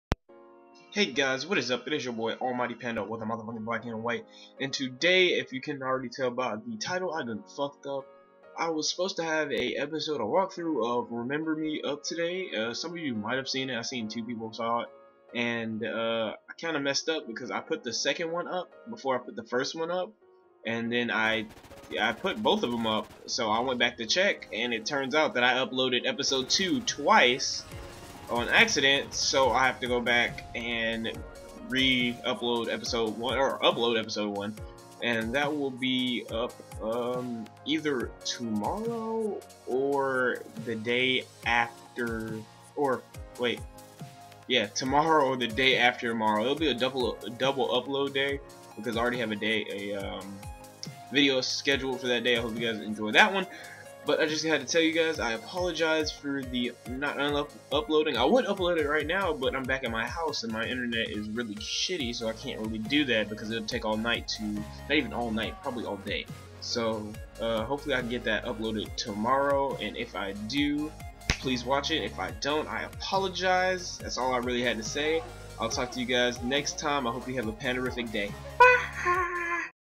I F*cked Up!
Hey guys sorry that i didnt upload today. The video that was scheduled to be uploaded to day will be uploaded tomorrow or the day after. If it is uploaded tomorrow it will be double upload so look forward to that! The second video will come out around 5:30 - 6. Hope you guys have a pandarifc day ByeHiiiiii!